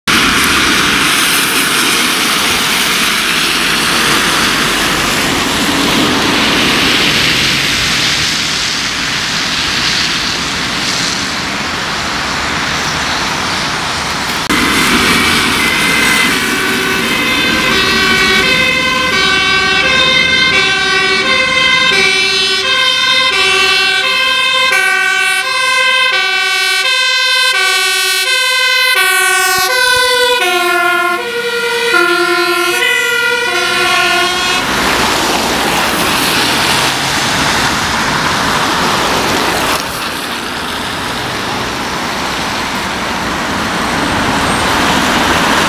Zwijndrechtse brandweer & Politie met spoed naar een mogelijke gaslekkage in Hendrik-Ido-Ambacht
In deze video is te zien:
00:00 Politie Vito [Bureau Papendrecht]
00:14 TS 186331 [Post Zwijndrecht]

Datum opname: 17/08/2021
Plaats opname: Langeweg, Zwijndrecht
Video opgenomen door: Claudius de Haan

-Copyright:
Op al mijn beelden met of zonder watermerk zitten auteursrechten.

-Sociaalmedia:
Ik ben ook te vinden op sociaalmedia: